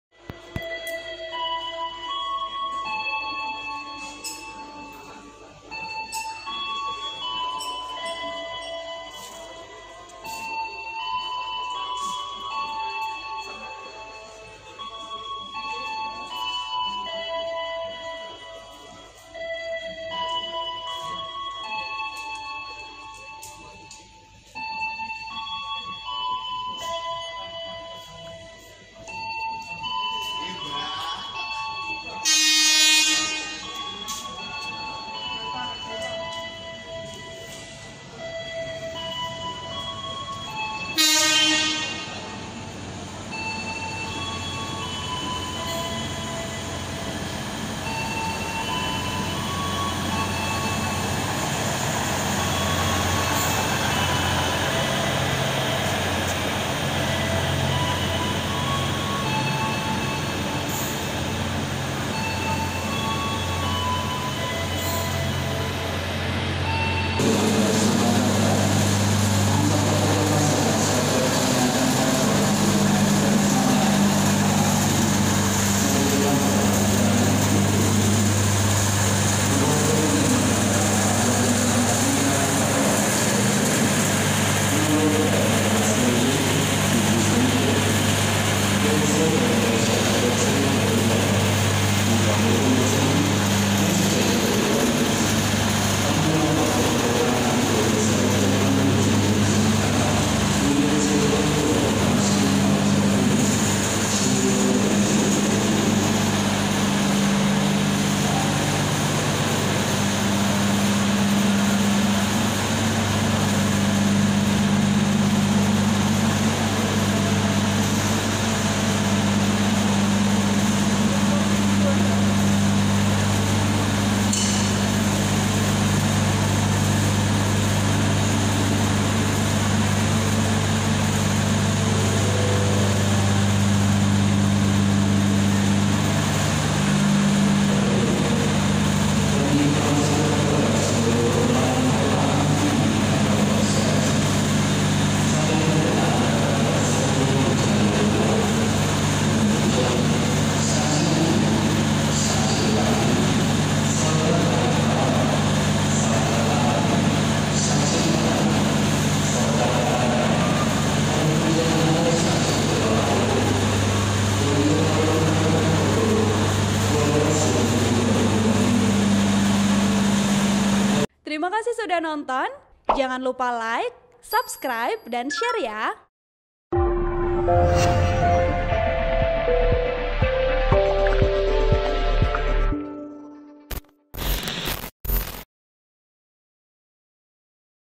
Kemenhub Dorong Reaktivasi Jalur Kereta Api di Sumatera Barat, Termasuk ke Sawahlunto
TRIBUN-VIDEO.COM - Kementerian Perhubungan (Kemenhub) melalui z

Sekretaris Direktorat Jenderal Perkeretaapian, Zulmafendi mengatakan salah satu jalur yang akan dibuka ialah jalur kereta api Naras, Kota Pariaman menuju Sungai Limau, Padang Pariaman, Sumbar.

"Ada rencana reaktivasi jalur kereta api Naras - Sungai Limau pada 2023," kata Zulmafendi di Padang, Senin (15/11/2021).(*)